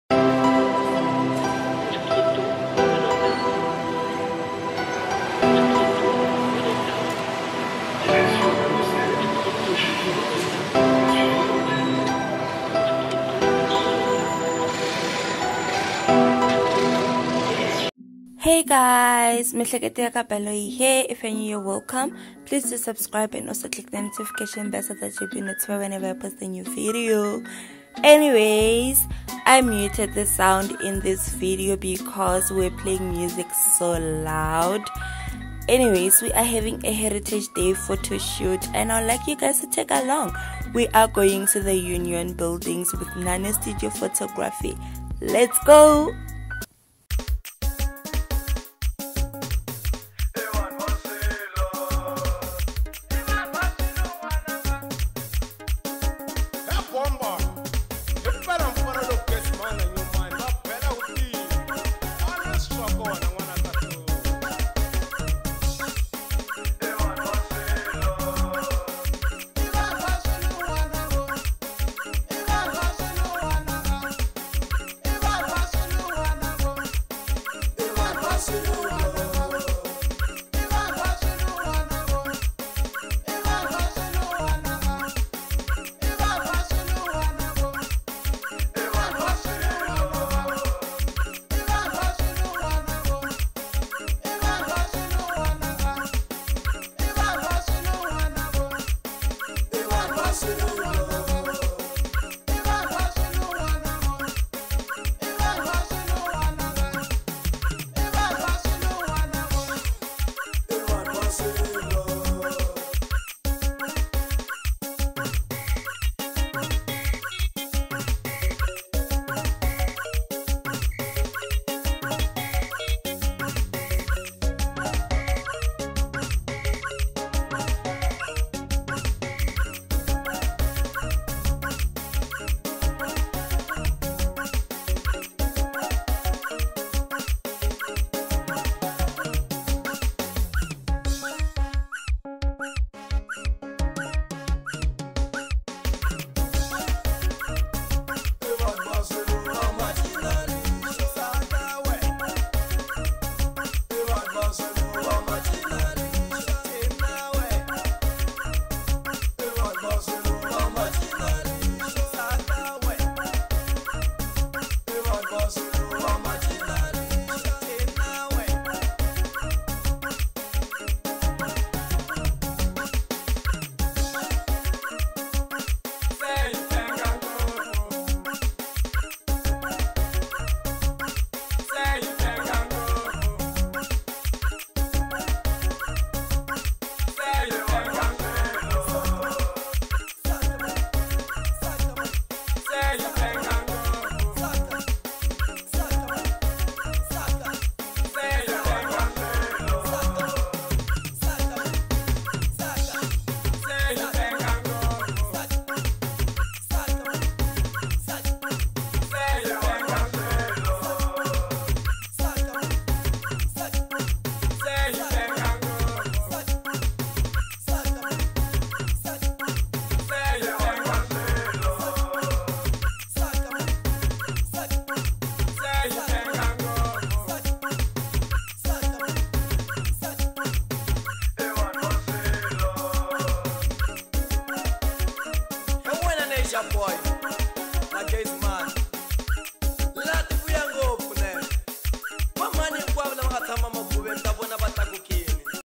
MINI VLOG || HERITAGE DAY PHOTOSHOOT WITH NANO STUDIO PHOTOGRAPHY - UNION BUILDINGS
Having a mini heritage day photoshoot with Nano Studio Photography

They're located in Gauteng but they can get to any location should you wish to have a shoot with them